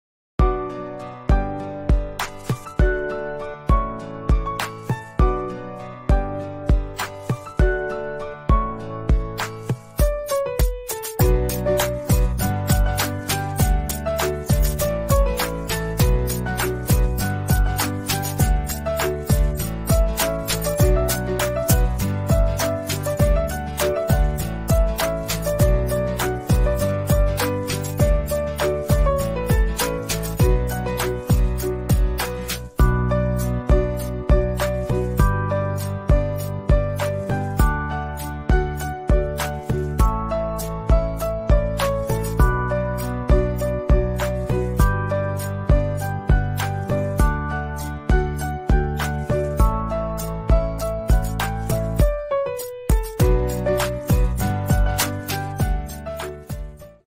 TOTALPOS H-D810L Kitchen 80mm Pos Thermal Receipt Printer with Built-In Led
1.Built-in LED and beeper to avoid order missing
2.Low Noise ,high speed up 300mm/sec
3.Reliable Auto cutter Life 1.5 million cuts
4.Support 58mm and 80mm paper printing
5.Long Printing head life 150 km, and high printing quality
6.Support 1D,QR CODE,pdf417 printing
7.Fast transmission with 100M for LAN
8.Printer paper-lack monitoring,back mark detecting
9.Partial/full option cutter
10.Driver available for Windows and Linux
11.Compatible with EPSON ESC/POS commands
12.Support Wall mounted